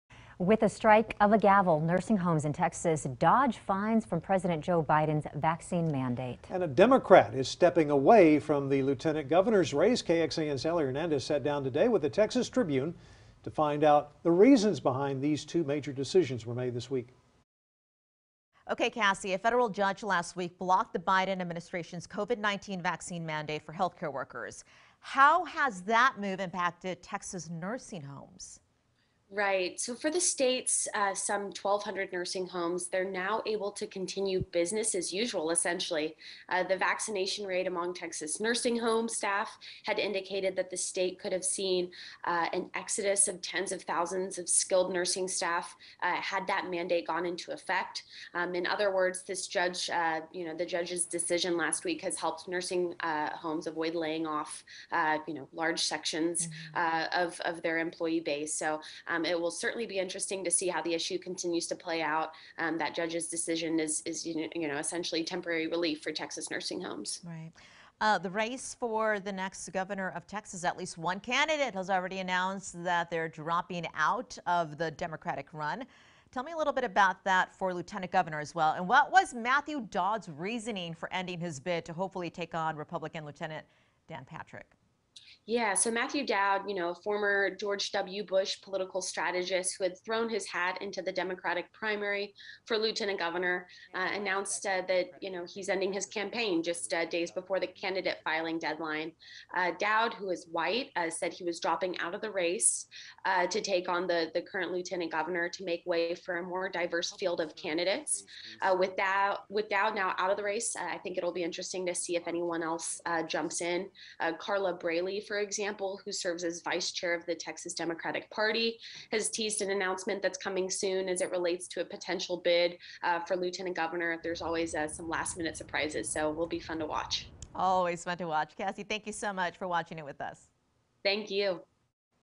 Texas Tribune crosstalk: Biden vaccine mandate, Lt. Governor's race
KXAN's Sally Hernandez sat down with the Texas Tribune to find out the reasons behind these two major decisions this week.